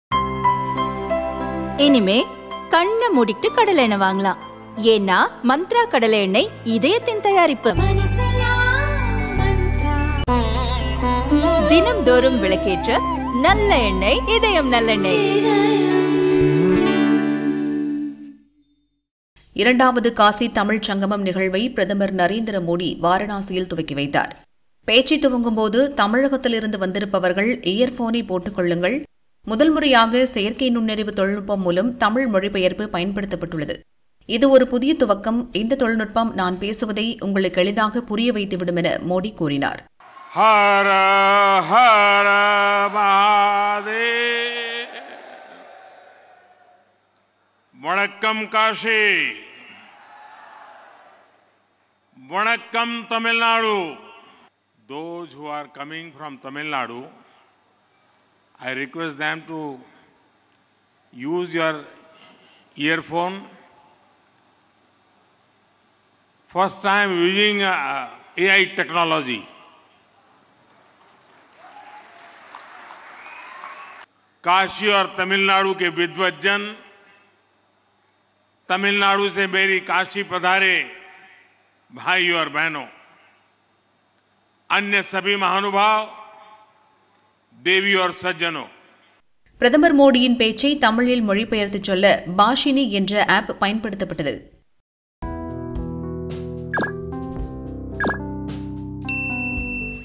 காசி தமிழ் சங்கமத்தில் முதல்முறையாக AI பயன்பாடு Modi | Kashi Tamil Sangamam AI technology Bhashini
மோடி பேசுவதை
தமிழில் மொழிபெயர்த்த
AI தொழில்நுட்பம்!

காசி தமிழ் சங்கமத்தில் முதல்முறையாக AI பயன்பாடு
Modi | Kashi Tamil Sangamam AI technology Bhashini#Narendra